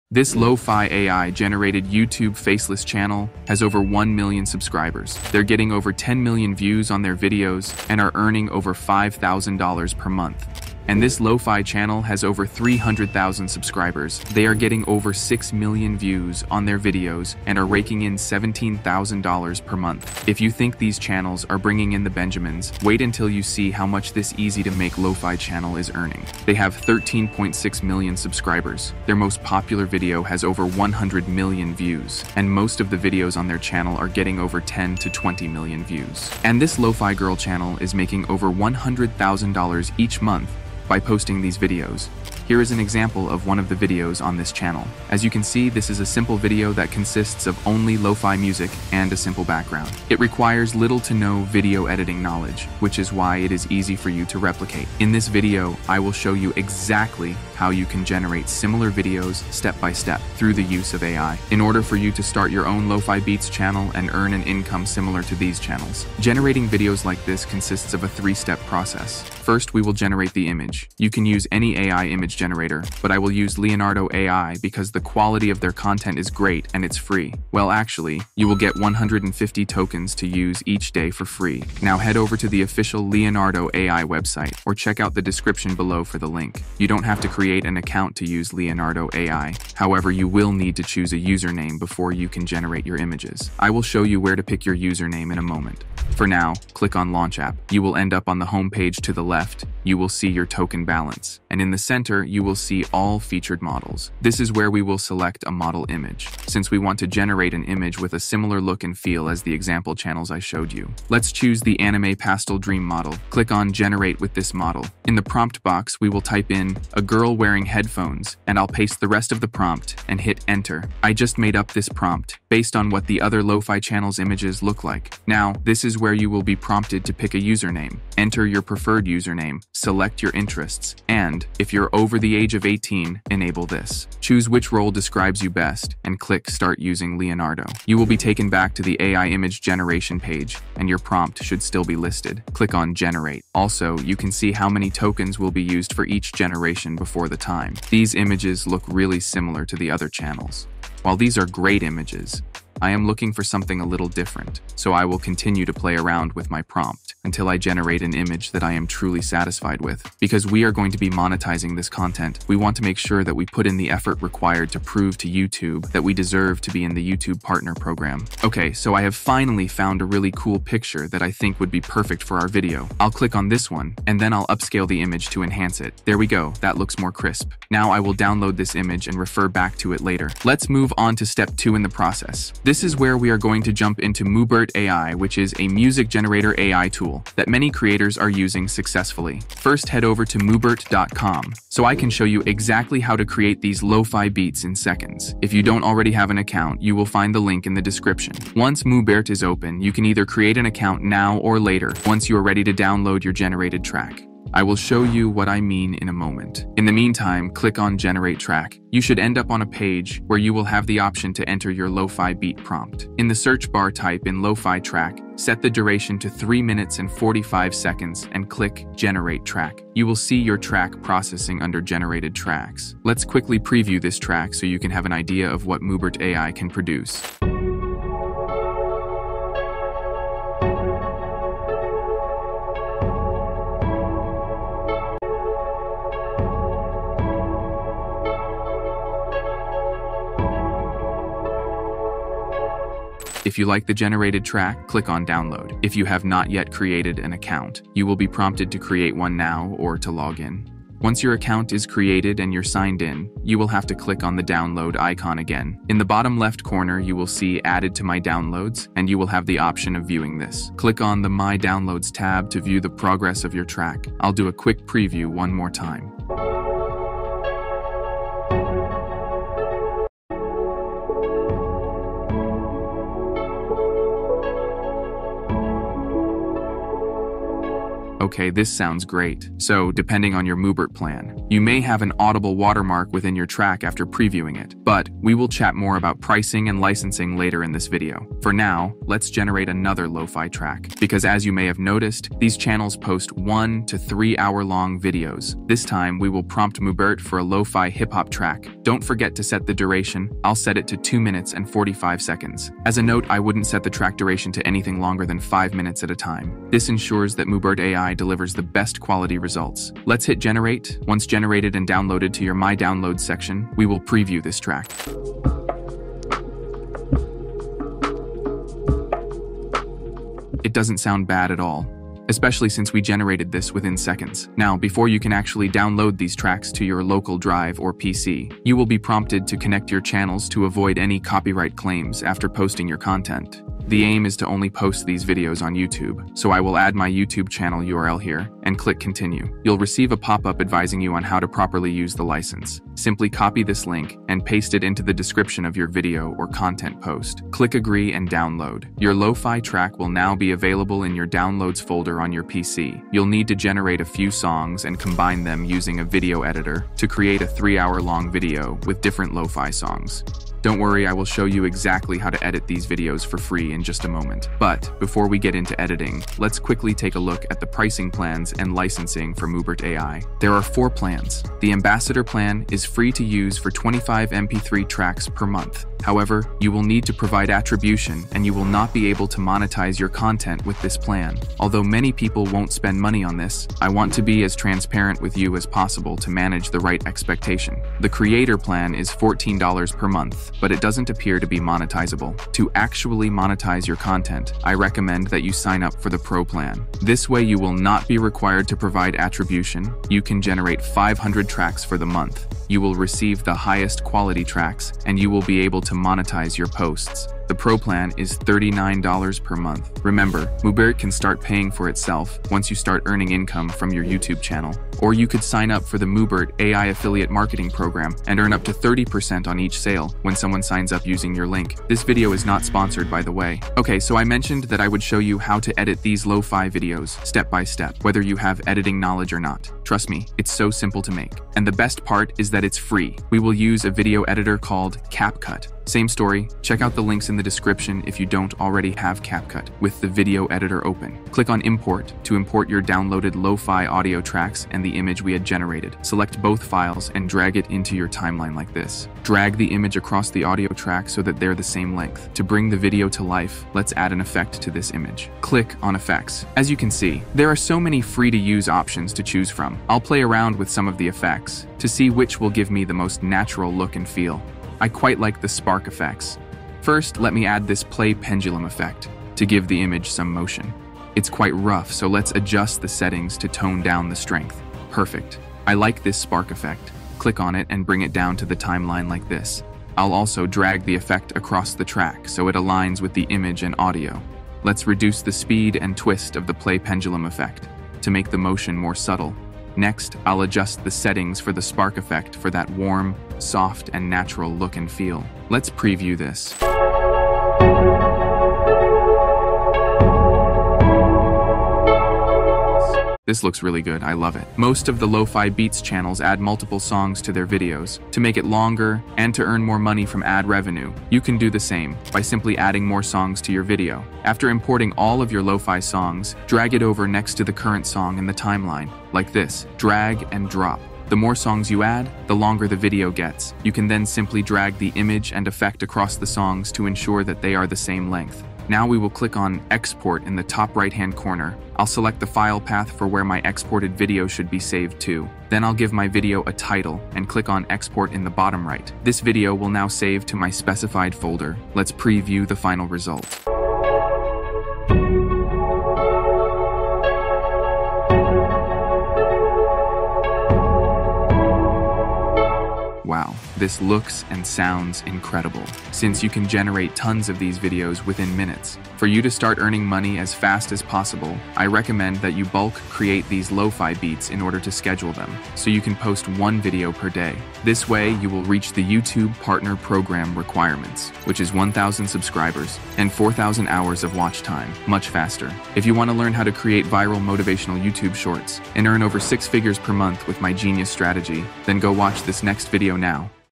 Start Your Own Monetizable LoFi Channel | Al Generated LoFi Beats | Step by Step Tutorial
😱 In this video 🎥 we will show you how to actually Start Your Own Monetizable LoFi Channel using Al Generated LoFi Beats Tools. 🤖

This video is a Step by Step Tutorial USING ONLY FREE AI TOOLS 🪄📲 so you can start earning money online TODAY! 💸 🌟📈

✅ TAGS - (Because apparently everyone uses them these days)